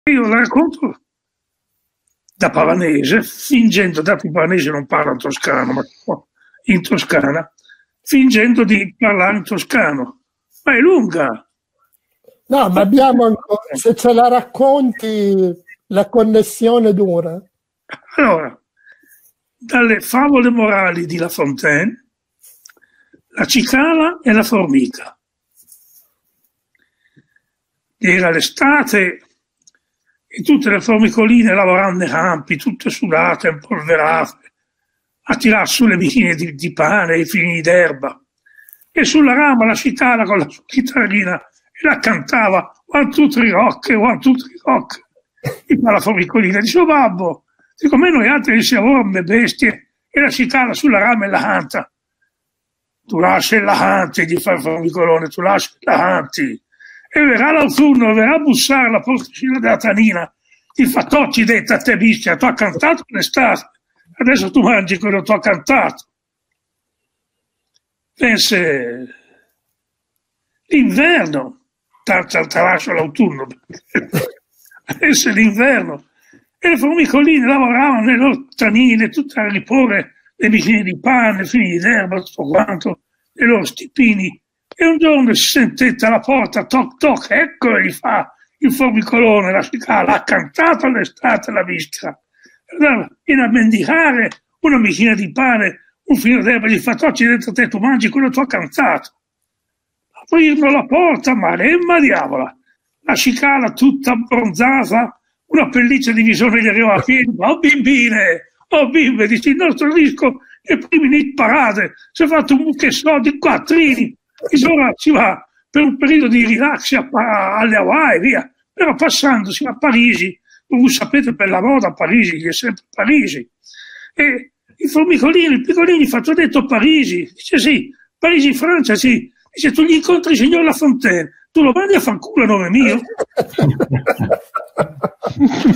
Gli 80 anni di Guccini, il maestro racconta in barzelletta 'La cicala e la formica' di La Fontaine
Grande raccontatore di barzellette, Francesco Guccini ama narrarle dal vivo. In una intervista in occasione dei suoi 80 anni ha detto che la più bella gliel'ha passata Baglioni, ma è lunga e non la può dire. In diretta per i festeggiamenti online del suo compleanno (che in realtà sarà il 14 giugno) il maestrone accetta invece di recitarla. E' la celebre favola di La Fontaine decicata alla cicala e alla formica, ma rivisitata in chiave comica. Da non perdere. Anche per sentire Guccini che con cadenza modenese imita l'accento toscano...